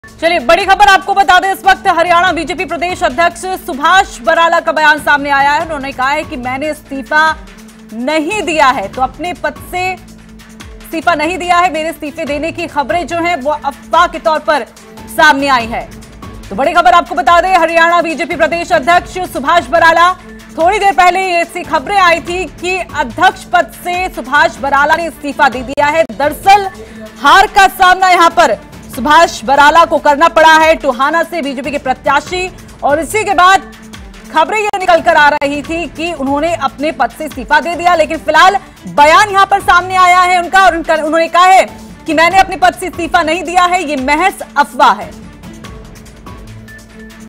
ELECTIONBREAKING : इस्तीफे को लेकर SubhashBarala ने दिया बयान
हरियाणा के बीजेपी प्रदेश अध्यक्ष सुभाष बराला का बयान सामने आया है...सुभाष बराला का कहना है कि मैने अपने पद से कोई इस्तीफा नहीं दिया है...उन्होंने कहा कि इस्तीफे की खबरें अफवाह हैं...